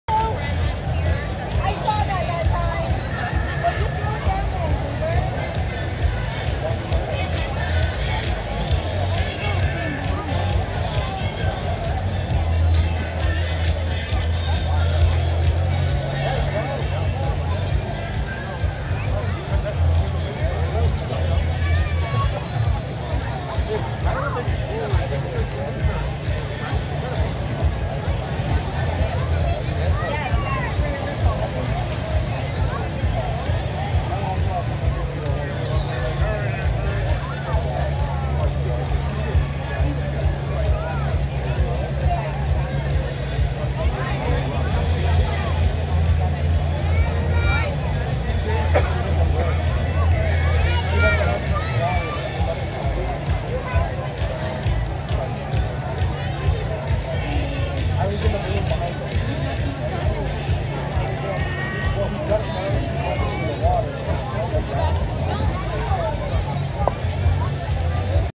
Great Falls Balloon Festival 2019 | Balloon Launch Part 2
Time for the Great Falls Balloon Festival Balloon Launch Part 2 of 3.